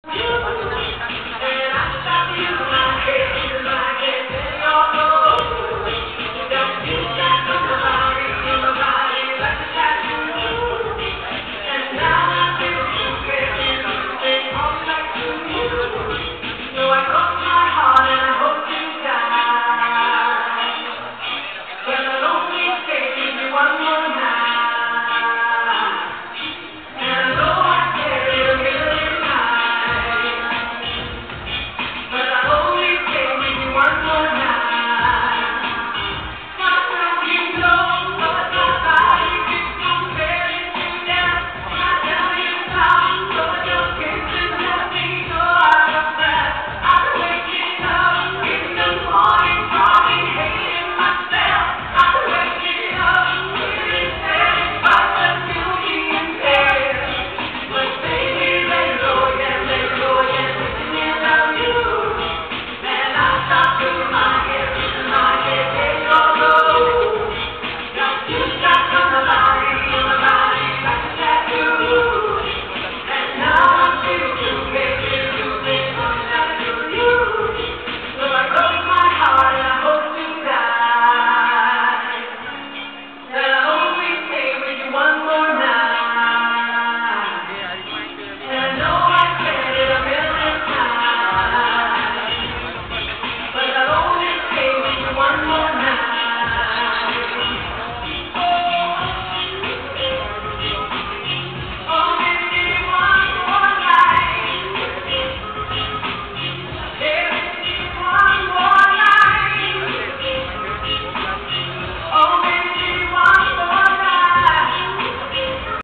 Pharmaniaga Dinner video 6
Pharmaniaga Dinner @ Royale Chulan Hotel on 23rd August 2014. Adibah Noor performance with 4  dancers